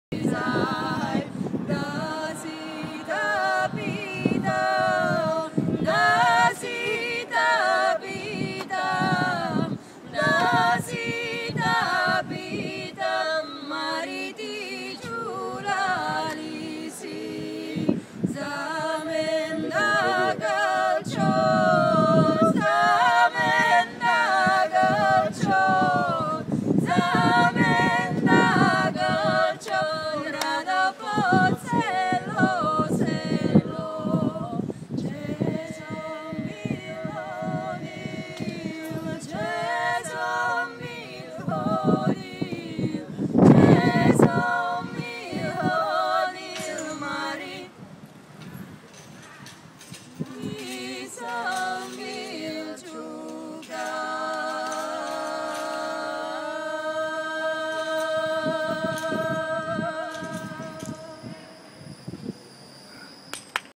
ThiasosInLisbon
May 20, 2011. In Lisbon. Members of Thiasos - Francesca Ferri, Camilla Dell'Agnola, Valentina Turrini out looking for a Fado club (which we eventually found!)